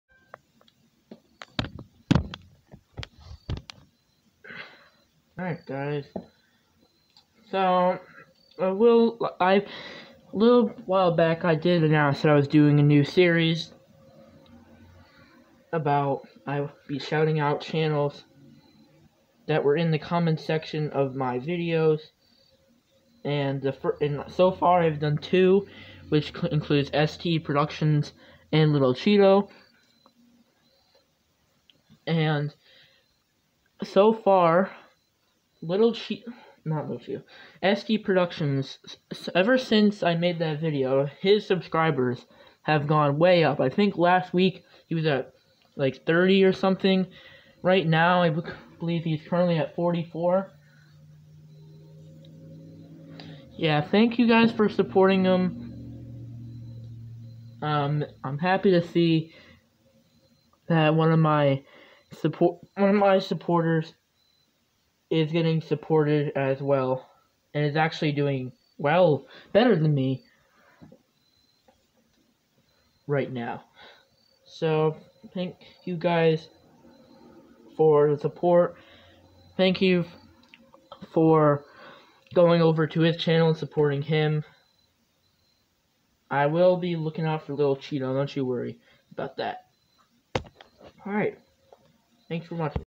Thank you for supporting ST Productions
Ever since I have made the video ST Productions subs have gone up like crazy.Last week he was at about 30 now he's almost 5 away from 50! Thank you guys for supporting him.